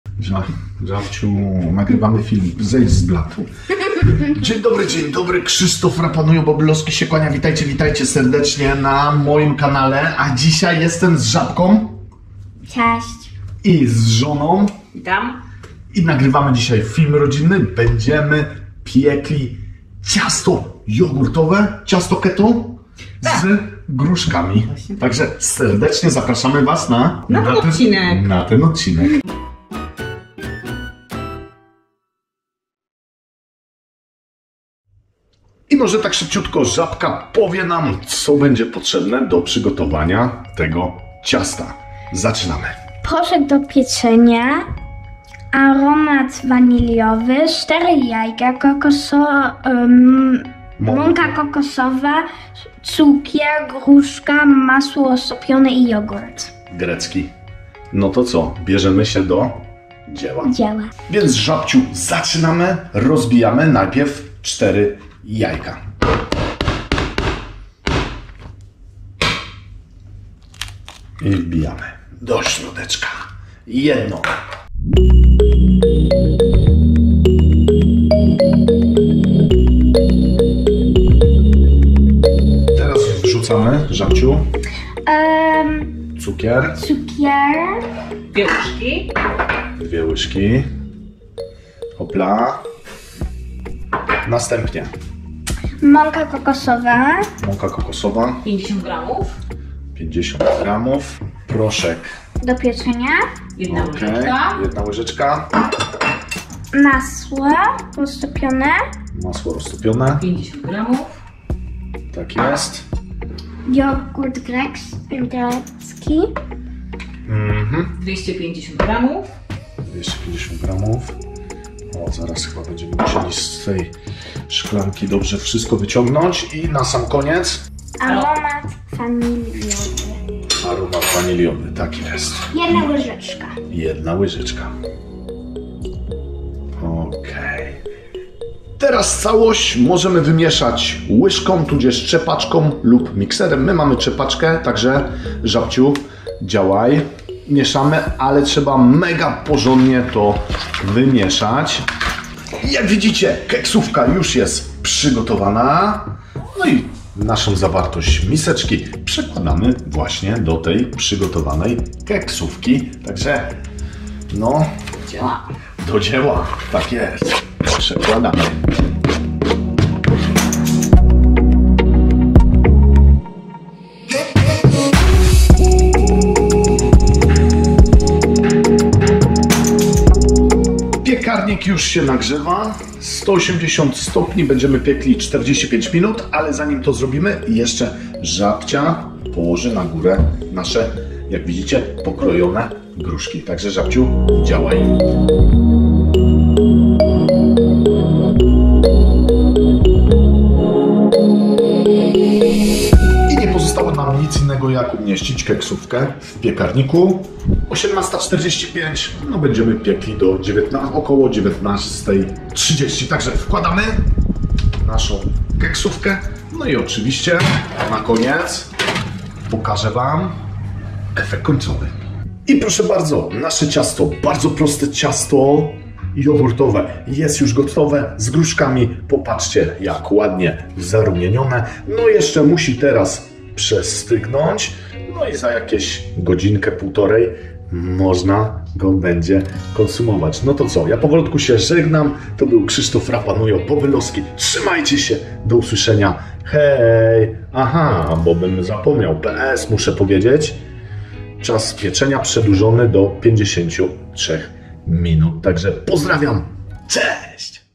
Ciasto jogurtowe z gruszkami!
Vloguś 419 Z cyklu: kuchnia dla laików: "" RapanujoNL @Rapanujo

Żeby przepis był w 100% keto użyjcie słodzika zamiast białego cukru i np truskawki zamiast gruszki!
Smacznego!

A ja będę się cieszył z tego jak berbeć w piaskownicy.  I nie zapominaj, że nowy dzień to nowe wyzwanie! Pozdrawiam serdecznie!

Smaker.pl: jako Rapa
Zajrzyj także na moje playlisty:-)